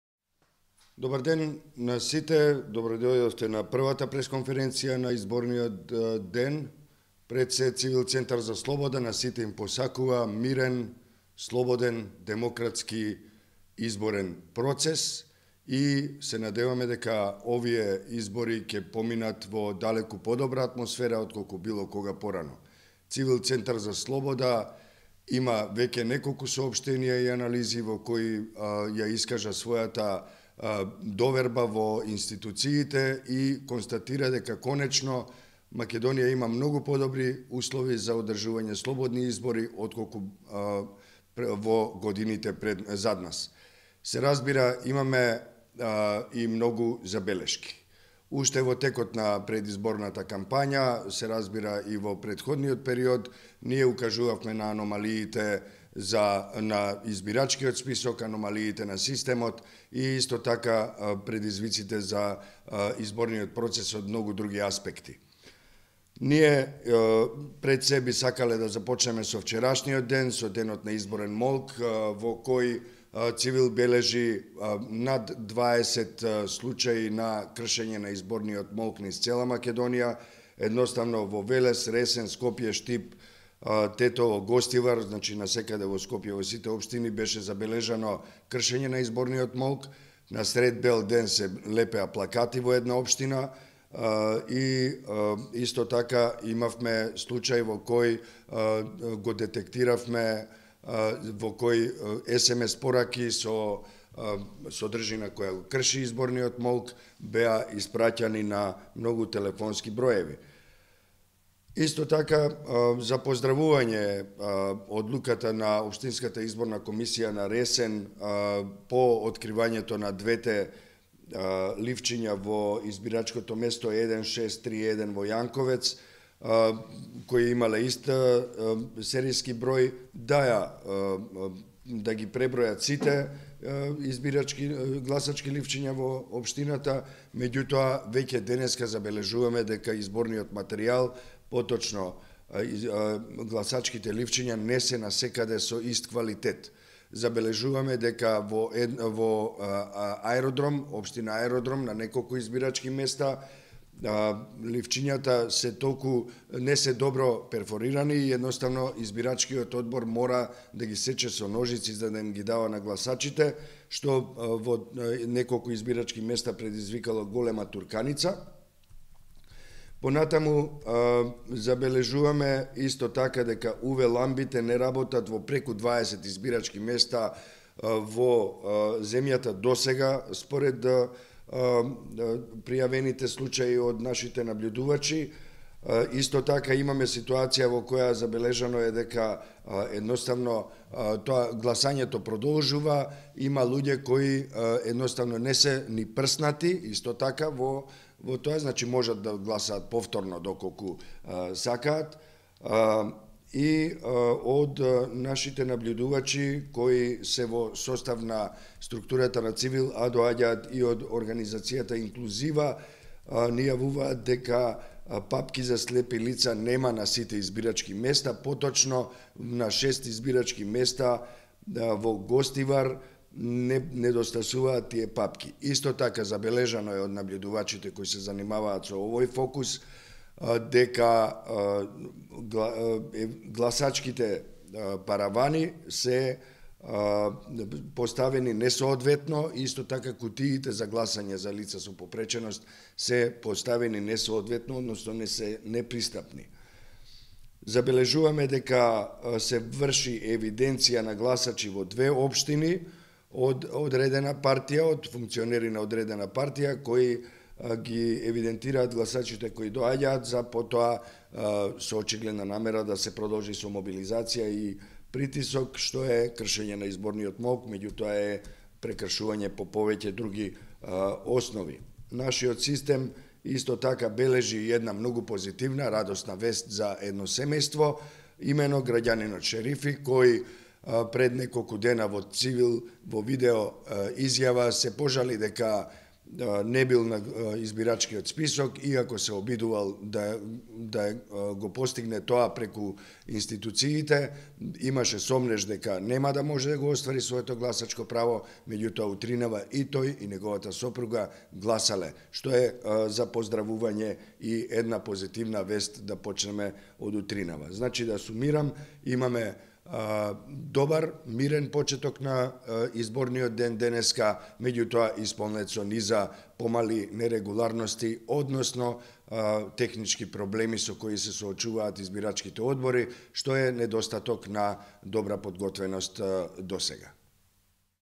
СлободниИзбори ЦИВИЛ прес во 0800 часот
Локални избори 2017
Прва прес конференција на ЦИВИЛ во 8 часот